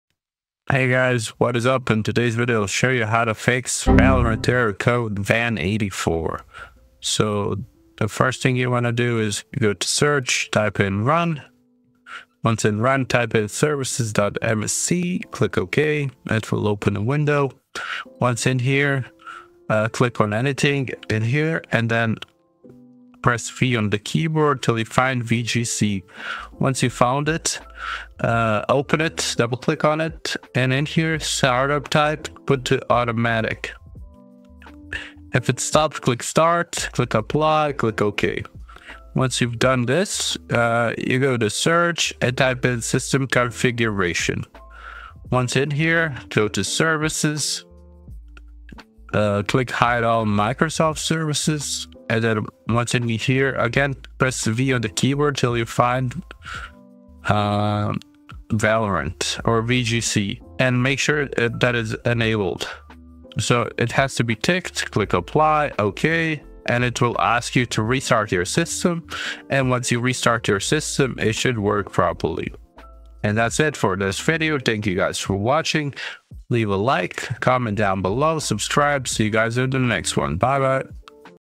HOW TO FIX VALORANT ERROR CODE VAN 84 | FIX VALORANT CONNECTION ERROR (2025)
In this video tutorial, I will show you how to fix the Valorant error code VAN 84.